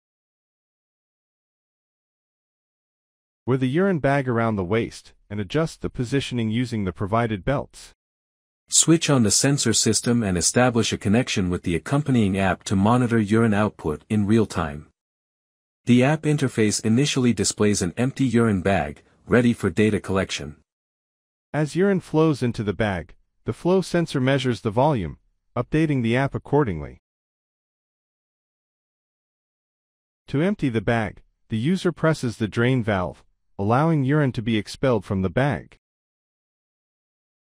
amSmartUrineBag
Urinary stoma (US) is the most common urinary diversion done in patients with bladder cancer who have their bladder removed. Urinary stoma is a prominent change in a patient’s body image, which they must live for rest of their life.  Although the creation of a stoma takes place in a tertiary care surgical context, long-term management is very often a primary care issue. World over there is only one system of application of a urostomy bag to collect urine, which needs to be emptied. These bags are not patient friendly and meet with many social and personal problems.

Following are the difficulties faced by the patient having a stoma
• Urostomy bags are hanging bag and impacts dignified living as their hanging bags are sign of their health issues.
• Protuberance is a major factor due to which patients face social embarrassment.
• Patient need to continuously physically monitor the bag to check for emptying if filled.
• Emptying of the pouch is not a one hand operation.
• A full pouch may hinder with any kind of physical activity.

The solution to the problem
The urostomy bag, which is designed by UnivLabs has unique features and more convenient in use. UnivLabs is designing “am Smart Uro-wear” which consist of mainly three parts-

1. Uro Pouch- Segmented pouch prevents protuberance
2. Electronics- Urine volume measurement.
3. Mobile application- Embedded with electronics for volume display along with AI algorithm to help patient empty pouch.
4. Pouch becomes part of body and not exposed, thus looks normal and avoids social stigma.